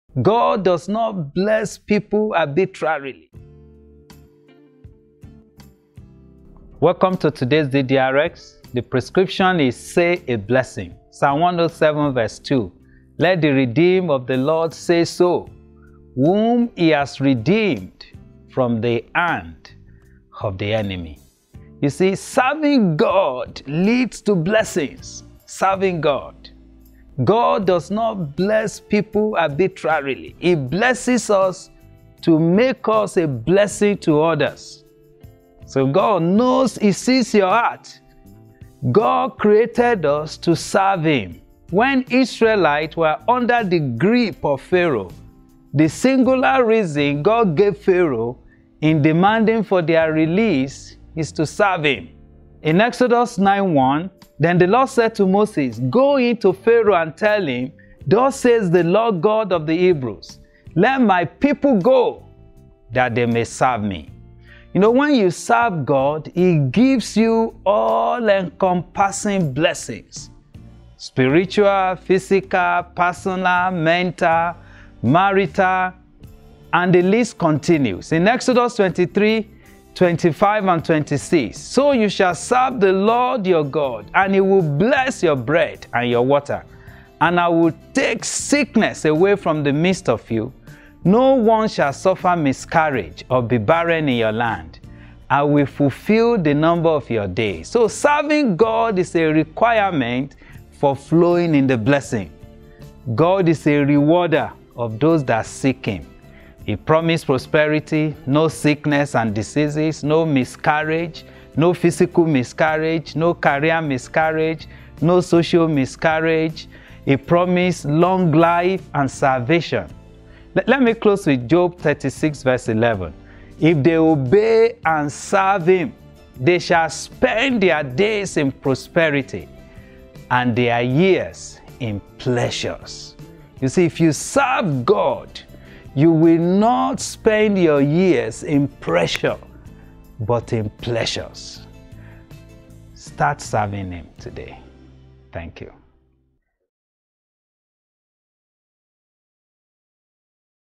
DDRxS30E06: Say A Blessing || Serving Precedes Blessings
God does not bless people arbitrarily! He blesses us to make us be a blessing to others

Stay in Touch with Us!